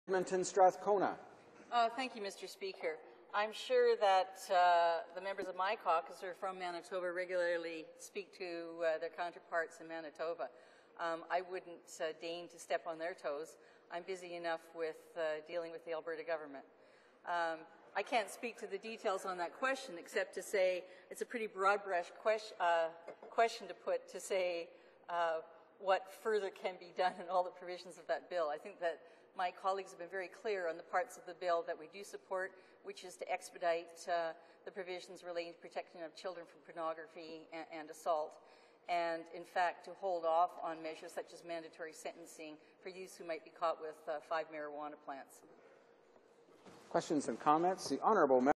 HoC Sitting No. 21 04:36:37 PM - Linda Duncan - Marijuana
Description
Sitting No. 21 House of Commons
Location
Centre Block - Chamber
Scheduled
Tuesday, Sep 27, 2011
10:00 AM - 7:00 PM
9 Hours
Actual
10:04 AM - 7:17 PM
9 Hours 13 Minutes

Mr. Speaker, I am sure the members of my caucus who are from Manitoba regularly speak to their provincial counterparts. I would not deign to step on their toes. I am busy enough dealing with the Alberta government.

I cannot speak to the details on that question except to say it is a pretty broad-brushed question to ask what further can be done in all the provisions of the bill. My colleagues have been very clear on the parts of the bill we do support, and that is to expedite the provisions relating to the protection of children from pornography and assault, and in fact to hold off on measures such as mandatory sentencing for youth who might be caught with five marijuana plants.
Safe Streets and Communities Act
Government Orders
September 27th, 2011 / 4:35 p.m.
NDP
Linda Duncan Edmonton Strathcona, AB